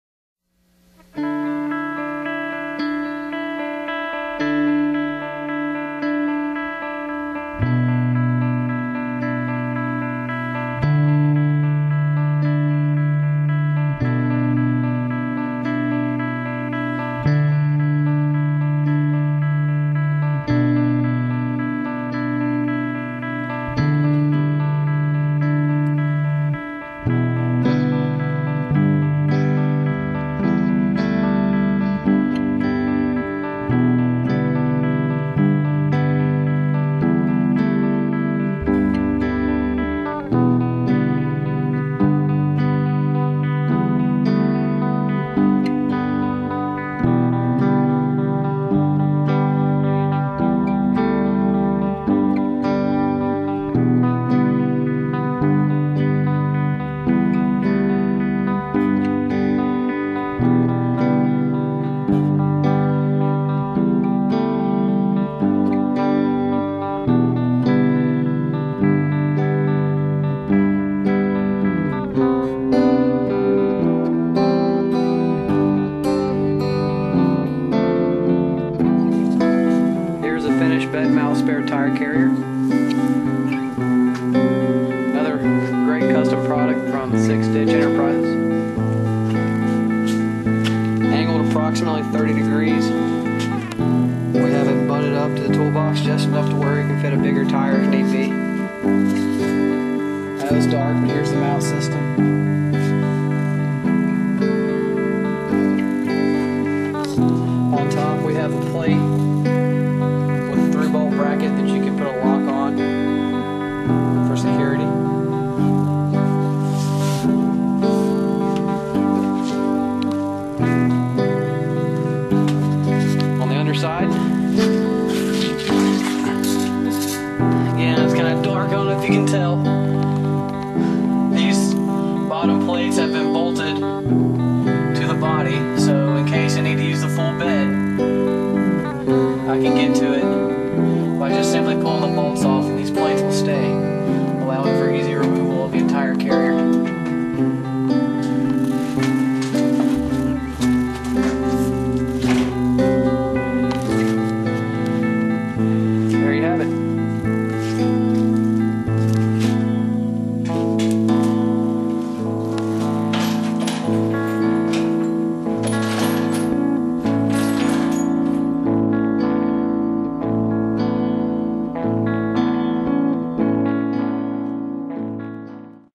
Toyota 4x4: Bed Mount Angled Spare Tire Carrier
Questions and comments always welcome!!  Here is the complete build process for the angled spare tire carrier.  It's designed to be heavy duty, lockable, and easily removable within 5 minutes (once tire is removed) for easy access to the full bed, while being just as easily re-installed.